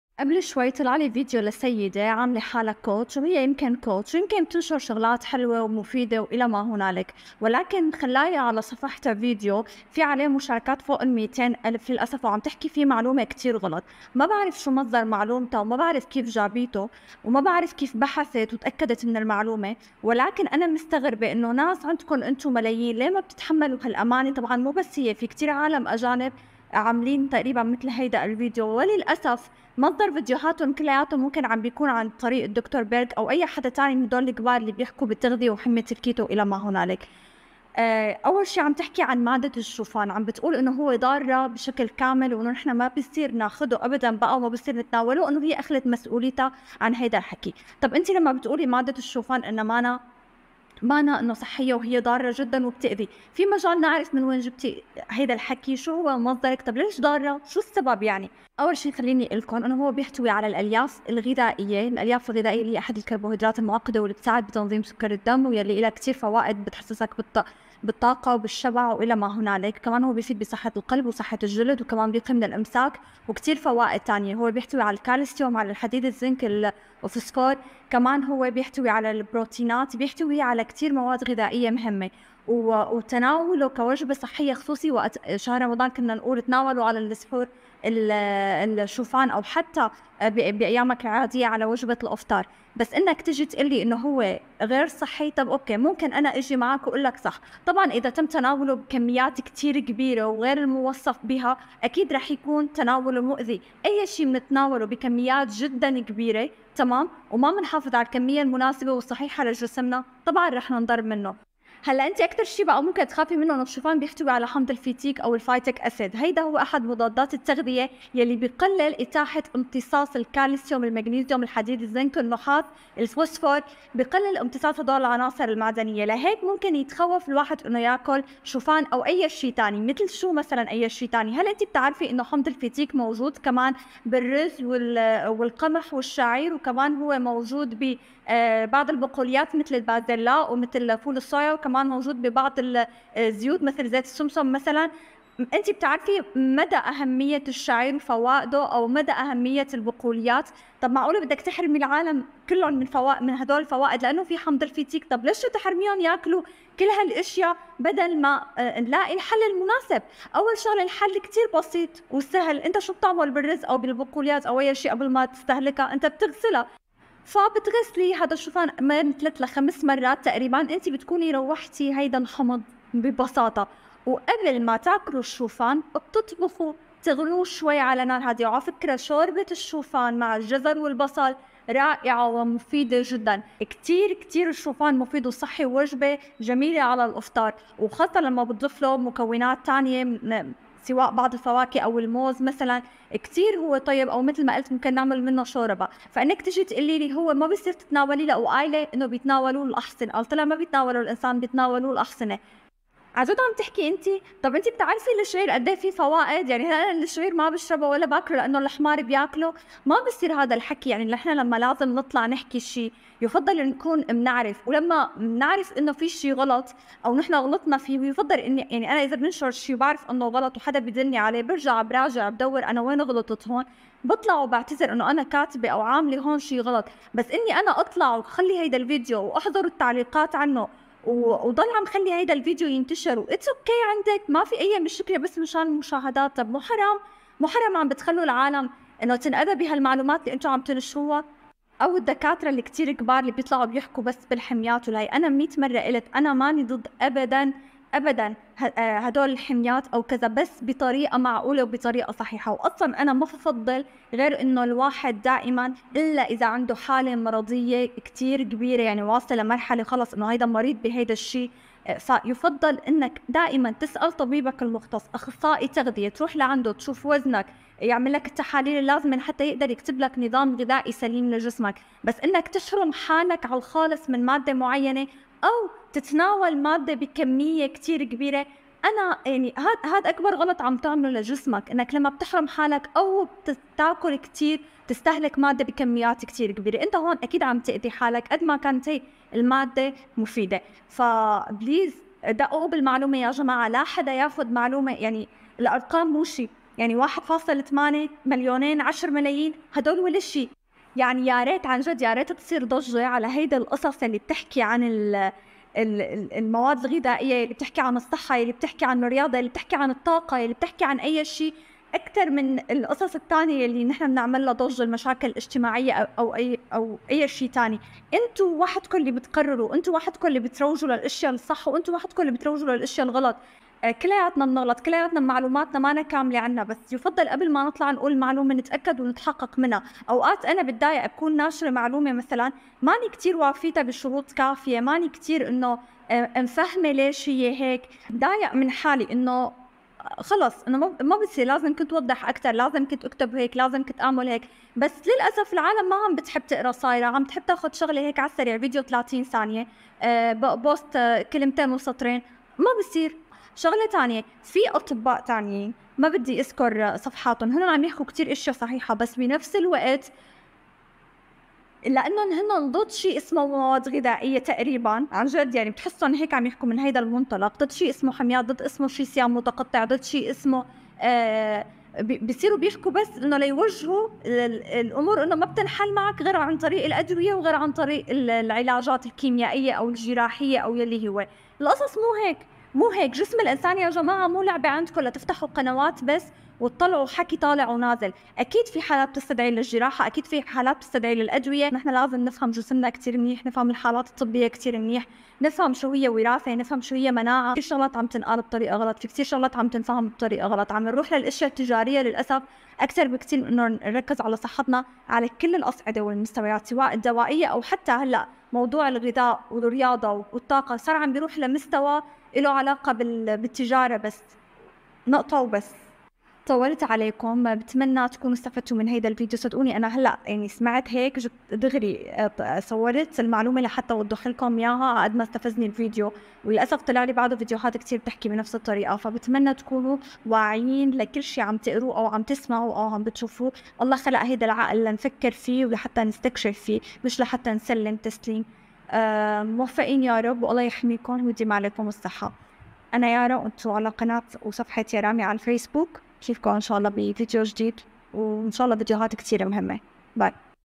حقيقية الشوفان | صحي أم ضار
الشوفان إله عدة أنواع وطبعا أفضلها ال organic, بيحتوي كمان على الافينين و lectins يلي متواجدين بالعديد من الخضار والمواد الغذائية فهل بيعني هاد نتخلى عن الشوفان.

شو هو المؤشر الجلايسمي..

كل رح نحكي عنه بفيديوهات قادمة

بحبكم كتير كتير ❤️❤️